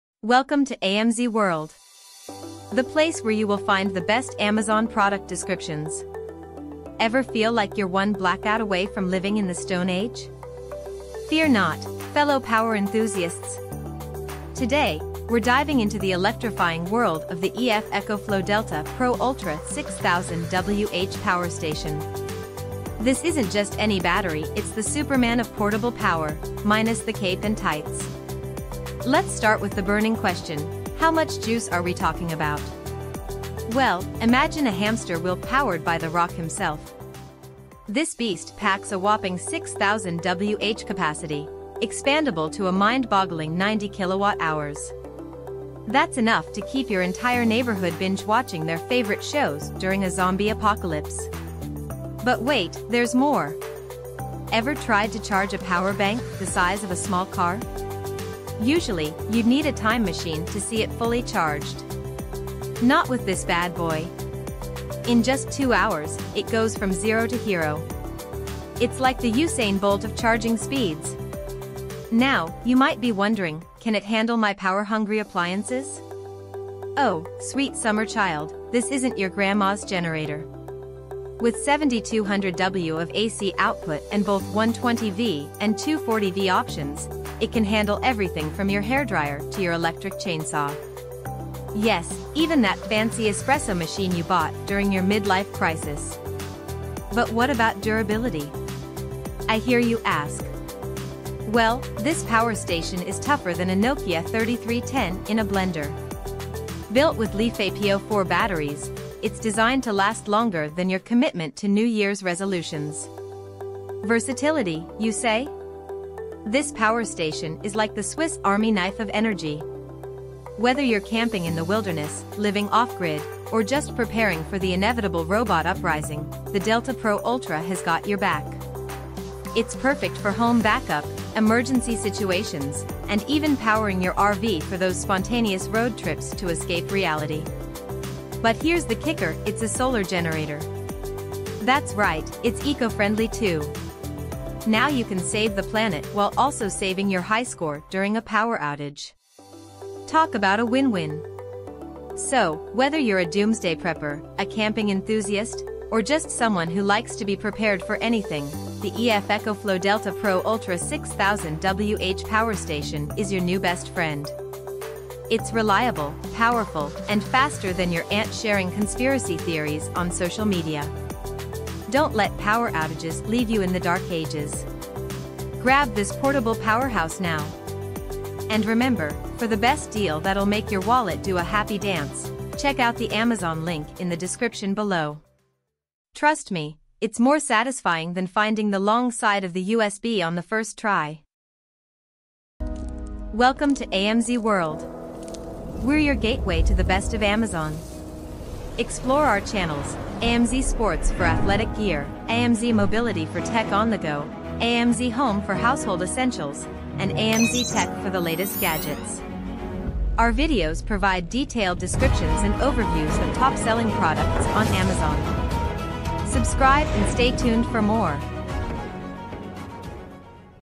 Describing EF ECOFLOW DELTA Pro Ultra 6000Wh Power Station, Amazon
Let me break down the key features mentioned:

Brand: EF ECOFLOW
Model: DELTA Pro Ultra
Capacity: 6000Wh (Watt-hours)
Output:

120/240V AC
7200W (Watts)

Battery type: LiFePO4 (Lithium Iron Phosphate)
Expandability: Can be expanded up to 90kWh
Charging time: 2 hours to full charge
Use cases: Home battery backup, emergency power, camping, RV use
Type: Solar generator (though it can likely be charged through other means as well)

This power station is designed to provide a significant amount of portable power. Its 6000Wh capacity is quite large for a portable unit, and the ability to expand up to 90kWh suggests it can be integrated with additional battery packs for even more capacity.
The 7200W output is powerful enough to run multiple high-draw appliances simultaneously. The 120/240V output indicates it can handle both standard household appliances and higher-voltage equipment.
The LiFePO4 battery chemistry is known for its safety and longevity compared to other lithium-ion batteries.
The quick 2-hour full charge time is a notable feature, as many large-capacity power stations typically take longer to charge.
Would you like me to elaborate on any specific aspect of this power station?

Ever feel like you're one blackout away from living in the Stone Age? Fear not, fellow power enthusiasts! Today, we're diving into the electrifying world of the EF ECOFLOW DELTA Pro Ultra 6000Wh Power Station. This isn't just any battery - it's the Superman of portable power, minus the cape and tights.

Let's start with the burning question: How much juice are we talking about? Well, imagine a hamster wheel powered by The Rock himself. This beast packs a whopping 6000Wh capacity, expandable to a mind-boggling 90kWh. That's enough to keep your entire neighborhood binge-watching their favorite shows during a zombie apocalypse!

But wait, there's more! Ever tried to charge a power bank the size of a small car? Usually, you'd need a time machine to see it fully charged. Not with this bad boy! In just two hours, it goes from zero to hero. It's like the Usain Bolt of charging speeds!

Now, you might be wondering, "Can it handle my power-hungry appliances?" Oh, sweet summer child, this isn't your grandma's generator. With 7200W of AC output and both 120V and 240V options, it can handle everything from your hairdryer to your electric chainsaw. Yes, even that fancy espresso machine you bought during your mid-life crisis.

"But what about durability?" I hear you ask. Well, this power station is tougher than a Nokia 3310 in a blender. Built with LiFePO4 batteries, it's designed to last longer than your commitment to New Year's resolutions.

Versatility, you say? This power station is like the Swiss Army knife of energy. Whether you're camping in the wilderness, living off-grid, or just preparing for the inevitable robot uprising, the DELTA Pro Ultra has got your back. It's perfect for home backup, emergency situations, and even powering your RV for those spontaneous road trips to escape reality.

But here's the kicker - it's a solar generator! That's right, it's eco-friendly too. Now you can save the planet while also saving your high score during a power outage. Talk about a win-win!

So, whether you're a doomsday prepper, a camping enthusiast, or just someone who likes to be prepared for anything, the EF ECOFLOW DELTA Pro Ultra 6000Wh Power Station is your new best friend. It's reliable, powerful, and faster than your aunt sharing conspiracy theories on social media.

Don't let power outages leave you in the dark ages. Grab this portable powerhouse now! Trust me, it's more satisfying than finding the long side of the USB on the first try!